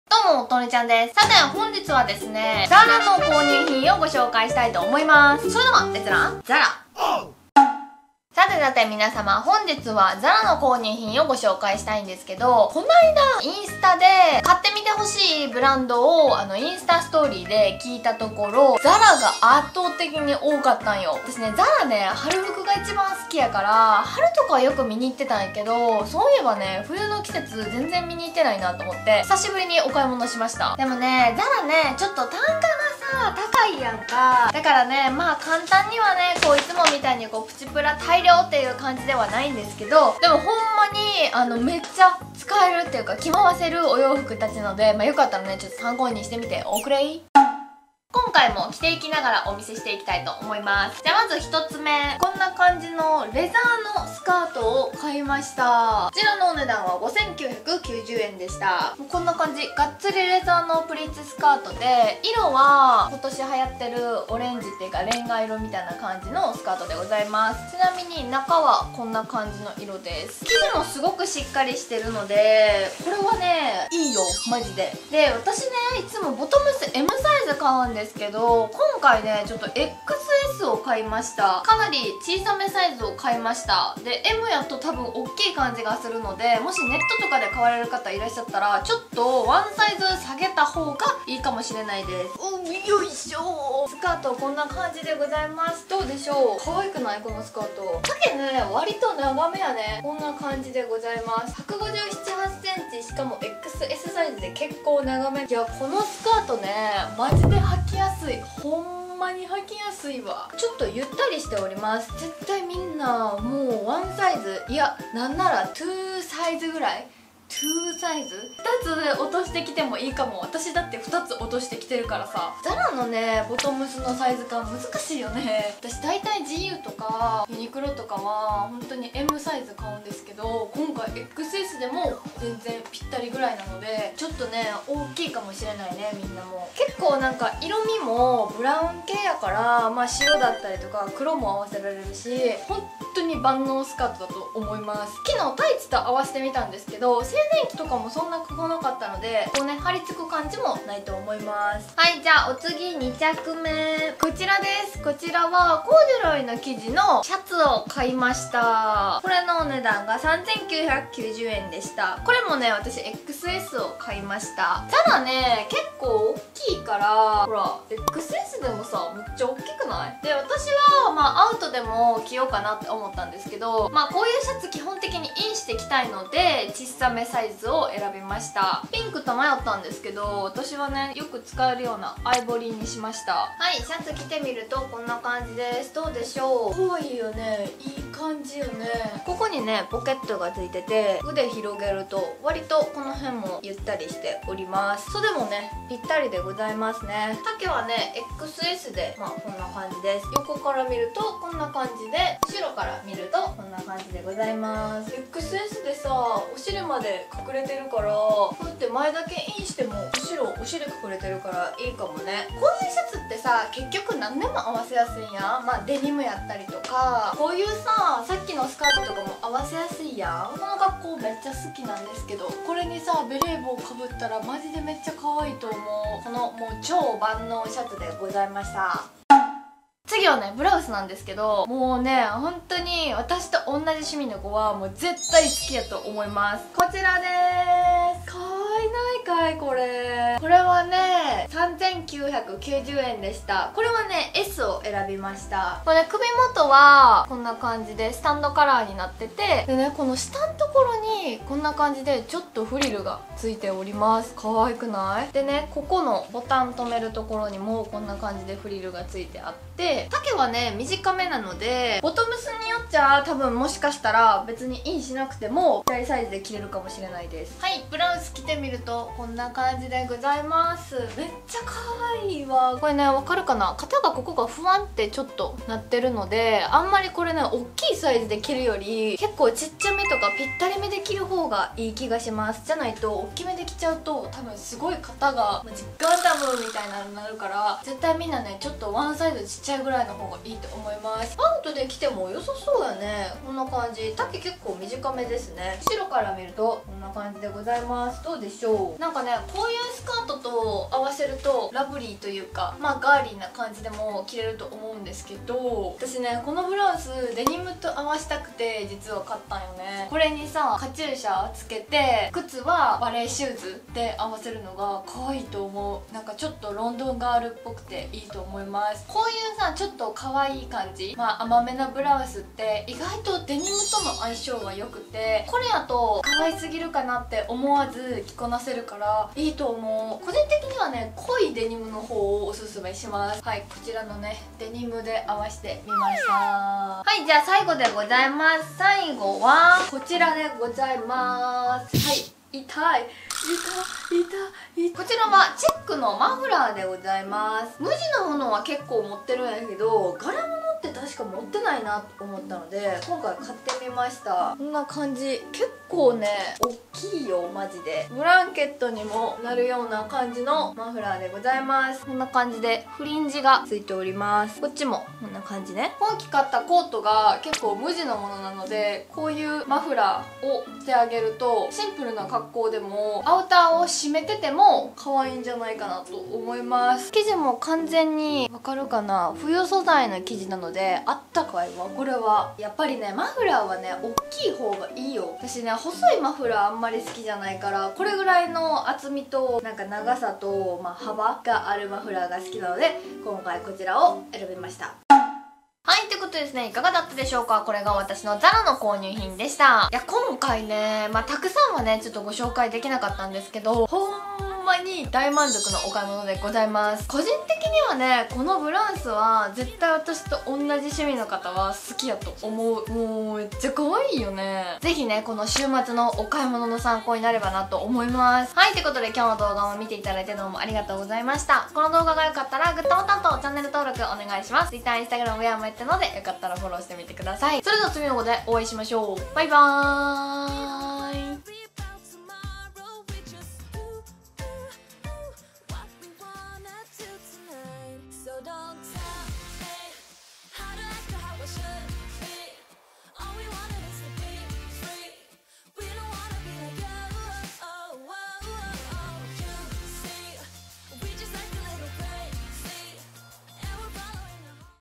【ZARA購入品】冬服が可愛いので外に出ようと思いました。可愛い冬服紹介するよ【2019冬】
今日も見てくださってありがとうございます！
🌼今日の服: 古着
デニム/ GU

【購入品詳細】
スカート

お仕事のご連絡は、こちらまでお願いします。